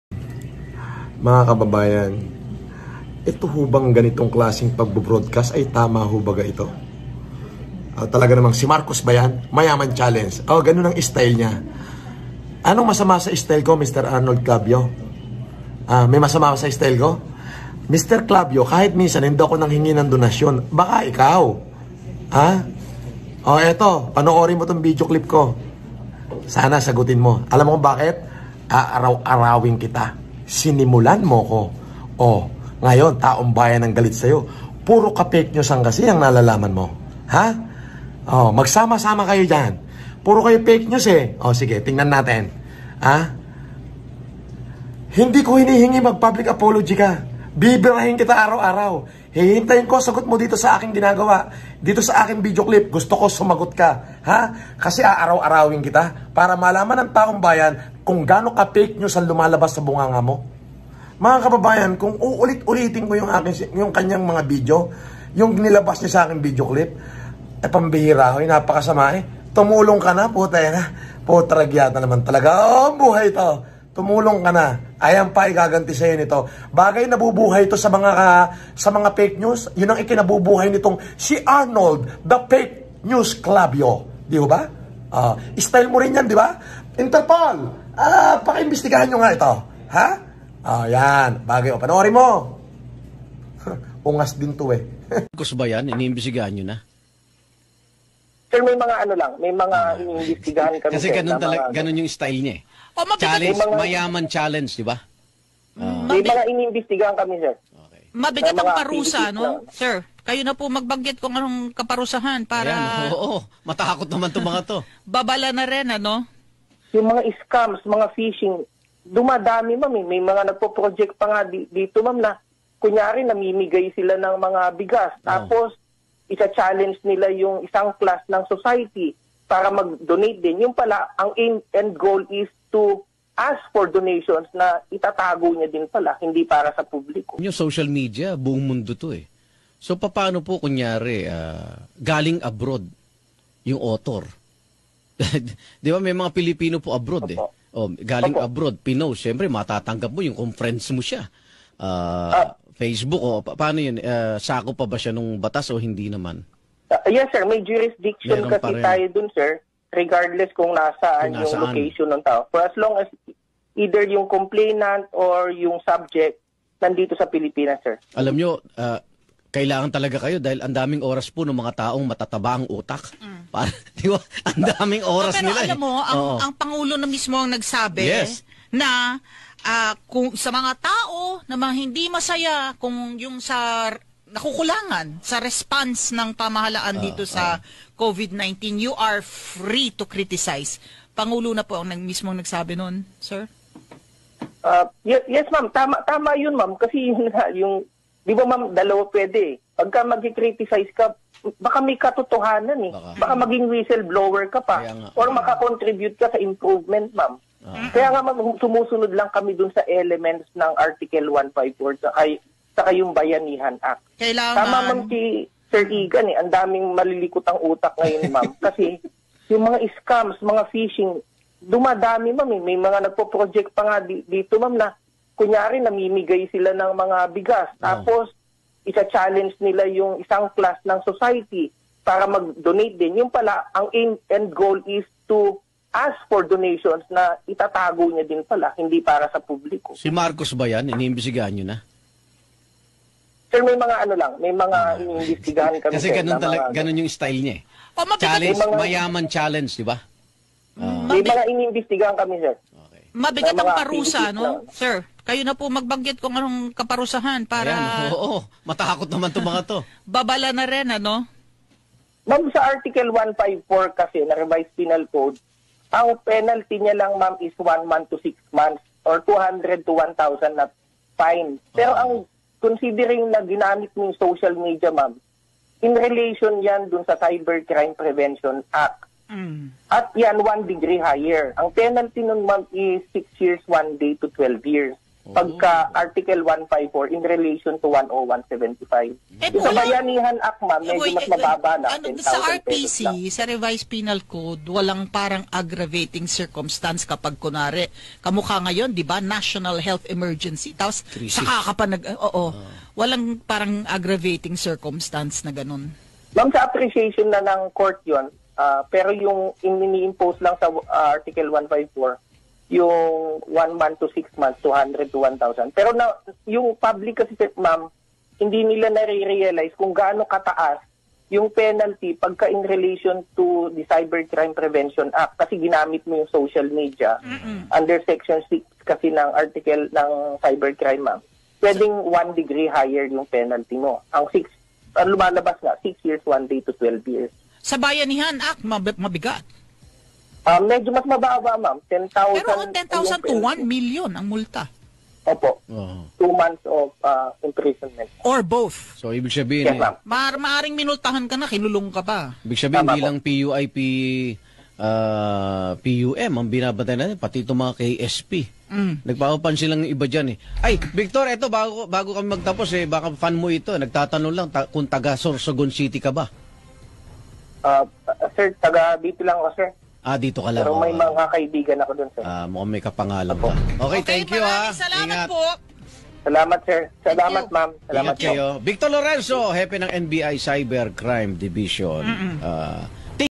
FLM BINANATAN NANAMAN SI ARNOLD CLAVIO! PANUORIN!
Hindi ko hinihingi magpublic apology mo! Bibirahin kita araw araw. Gusto ko sumagot ka!
Si arnold the fake news clavio nabubuhay sa fakenews! Ungas din to eh! - FLM